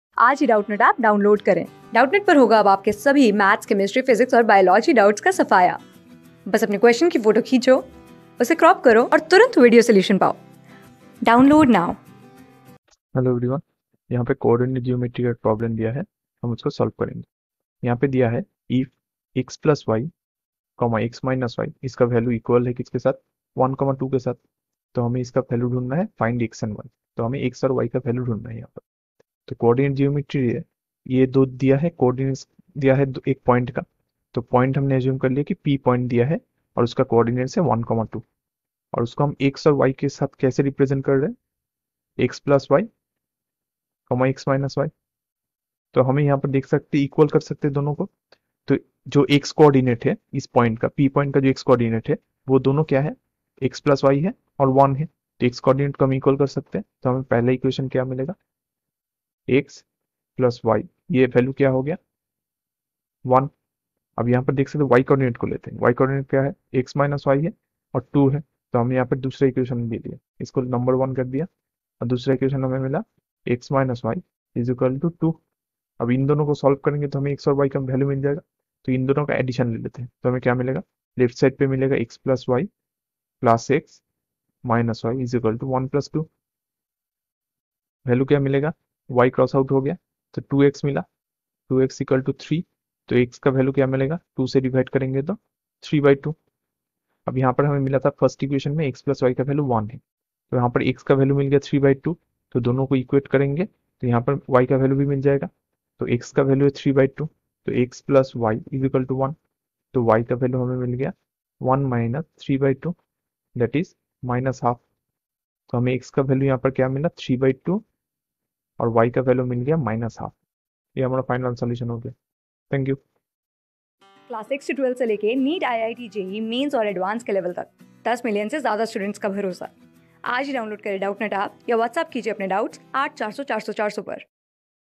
If (x+y, x-y)=(1,2), find x and y. | CLASS 12 | APPLICATION OF INTEGRALS | MATHS | Doubtnut
If (x+y, x-y)=(1,2), find x and y.
Class: 12
Subject: MATHS
Chapter: APPLICATION OF INTEGRALS
Board:Polytechnic

👉 Have Any Query? Ask Us.